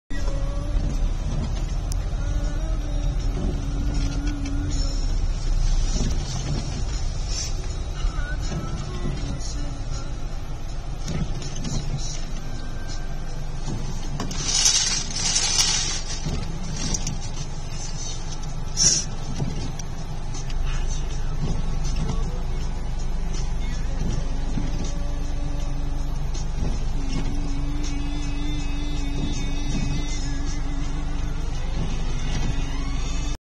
間の悪いワイパー
先日借りた代車のワイパーがおかしなタイミングでした。